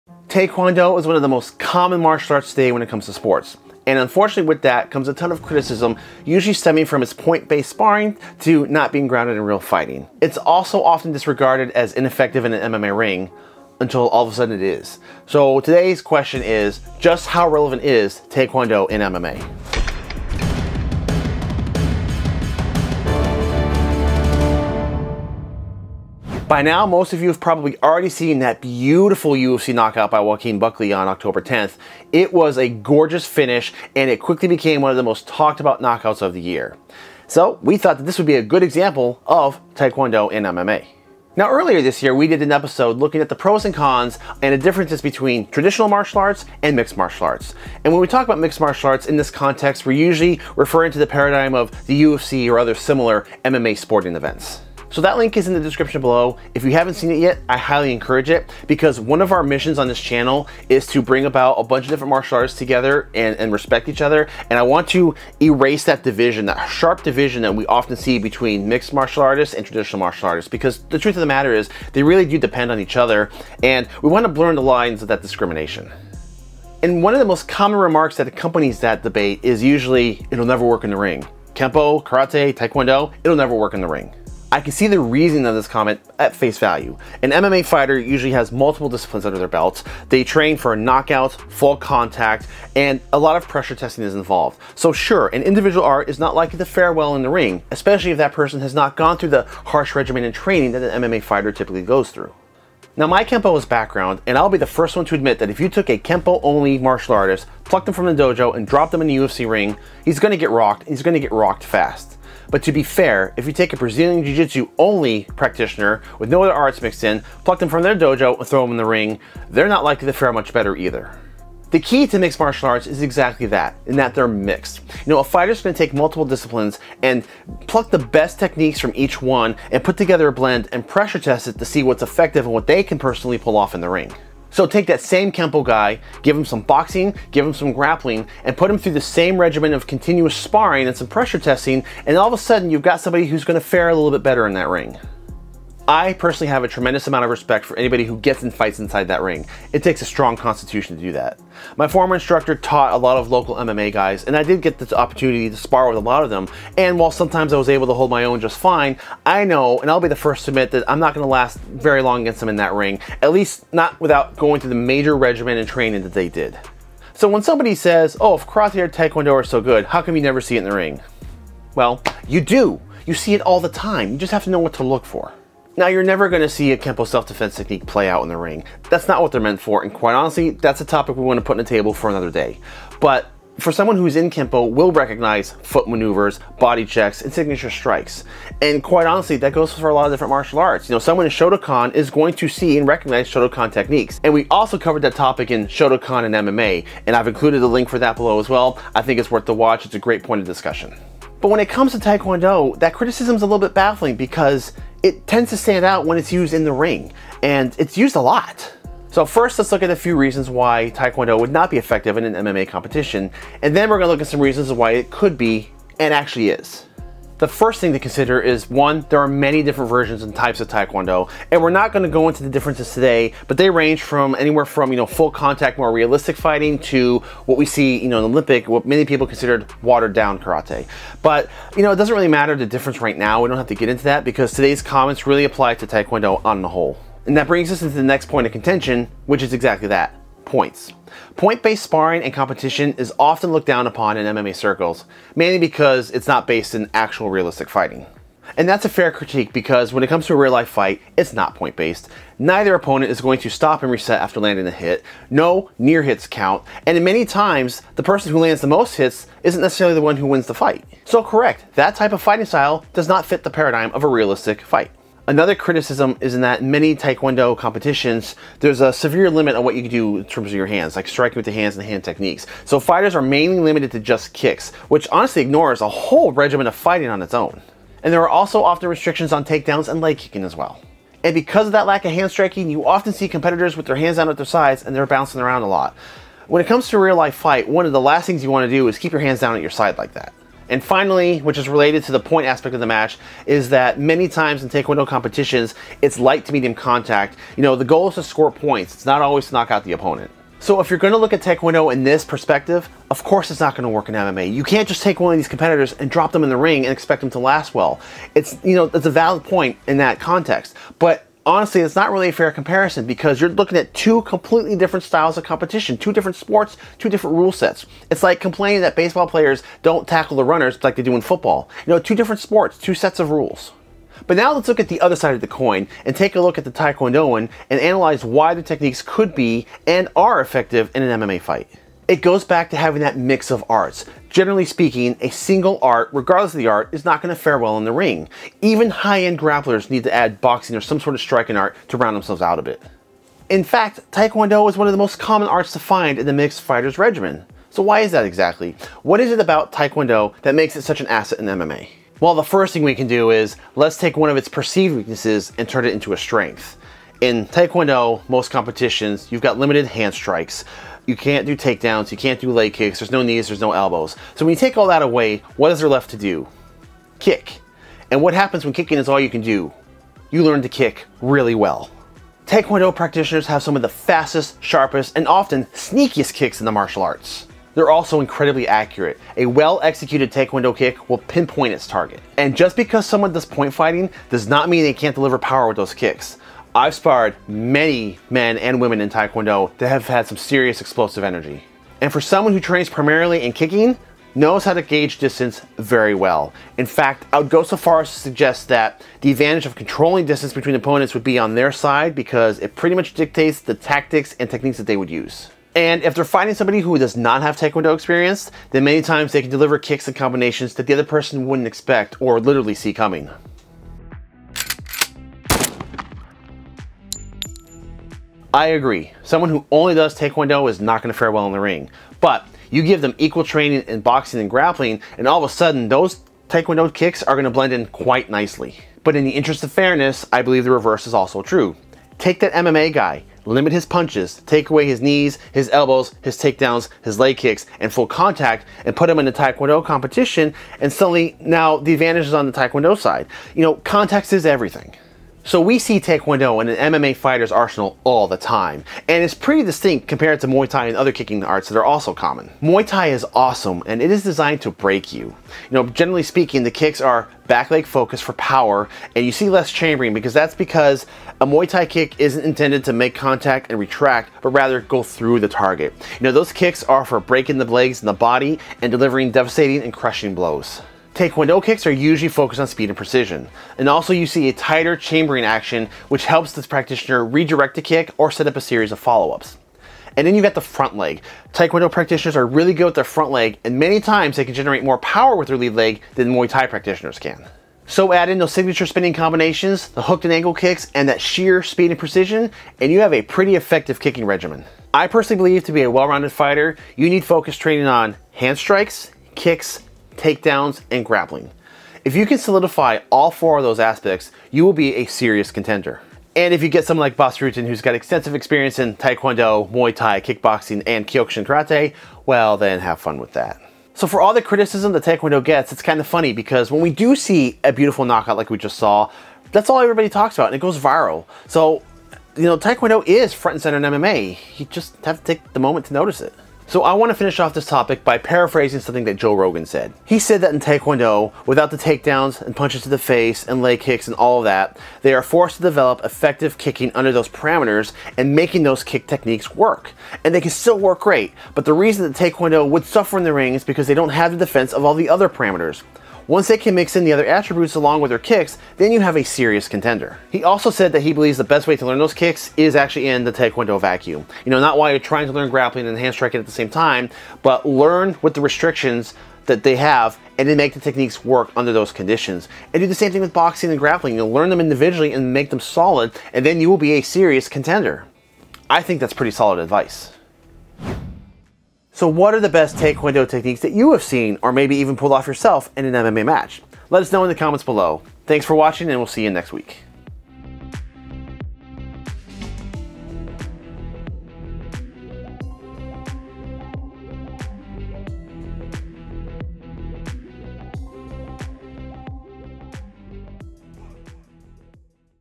Taekwondo in MMA? | ART OF ONE DOJO
"It won't work in the ring". The most common critique MMA fans say of Traditional Martial Arts. Taekwondo, Karate, Kenpo, are often at the forefront of this debate. Today we're going to take a look at Taekwondo, and examine it's role in the world of MMA and UFC fighting and if it really works in the ring or not. Let's examine Taekwondo in MMA.

◼️𝗔𝗖𝗖𝗘𝗦𝗦 𝗘𝗫𝗖𝗟𝗨𝗦𝗜𝗩𝗘 𝗠𝗘𝗠𝗕𝗘𝗥 𝗖𝗢𝗡𝗧𝗘𝗡𝗧! ◼️
(exclusive episodes, behind the scenes, seminars)

Video Production by Fade 2 Black Productions, Inc.